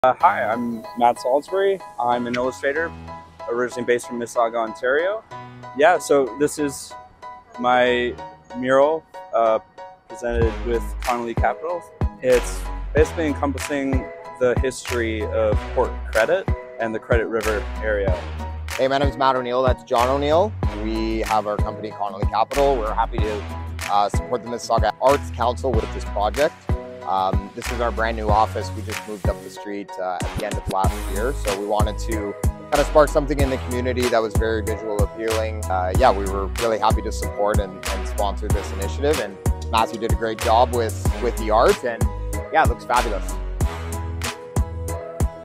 Matt's Port Credit Mural
🎉Artist Matt Salisbury created a beautiful mural which tells the story of the Port Credit community!
Artist: @salis_sketch !

Artist Statement:

This mural is made to reflect the long flowing history of Port Credit from its beginnings as a home to the Mississaugas of the Credit first nations to being a key trading post in 1750 and even to now being a place of comfort, all while holding true to the history of Connolly Capitals founders with their shared love of hockey and passion for the history of where their business calls home.

Location: 119 Lakeshore Rd W, Mississauga